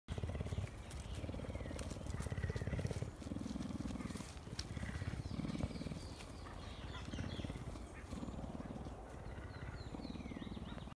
Pumas Purring at the Belize Zoo
A couple of pumas rolling around, walking around, and purring for the camera.